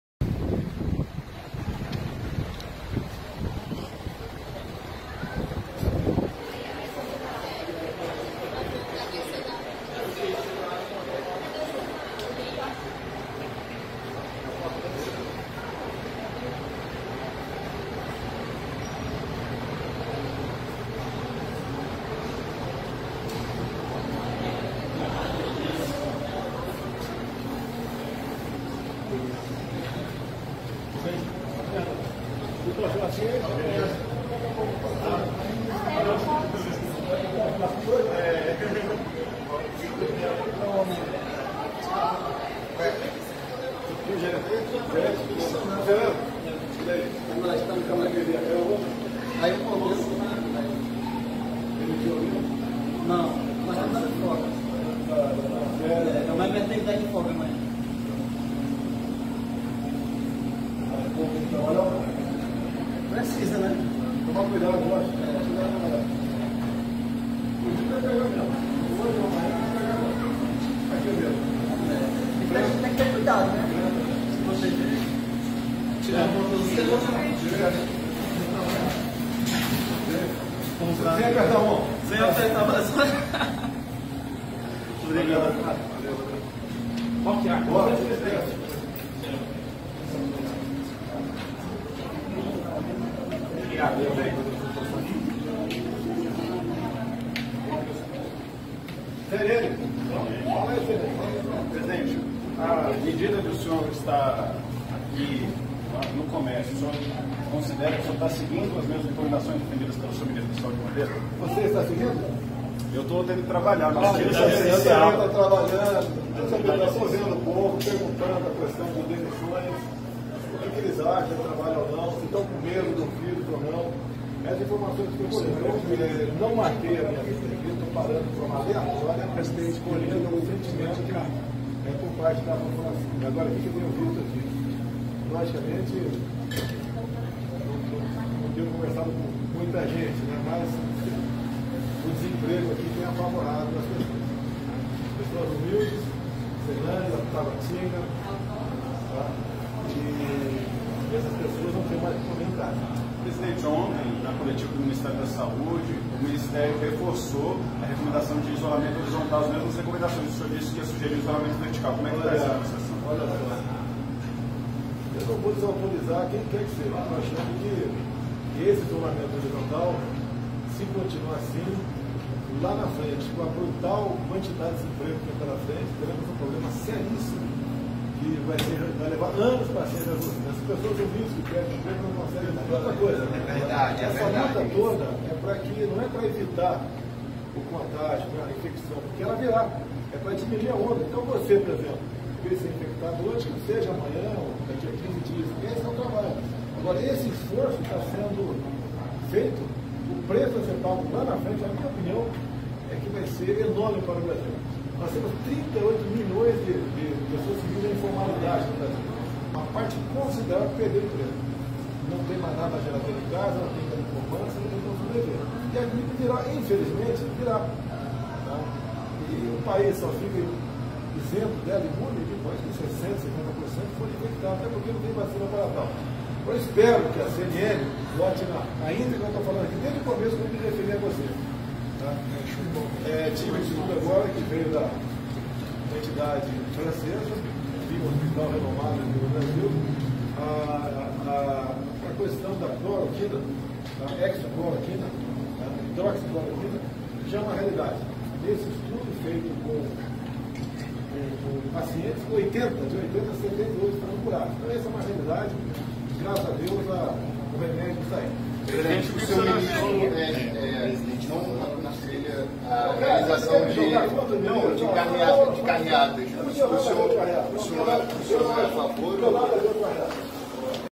Sobradinho/DF. Bolsonaro esteve também em sobradinho , conversando com várias apoiadores
Sobradinho/DF.

Bolsonaro esteve também em sobradinho , conversando com várias apoiadores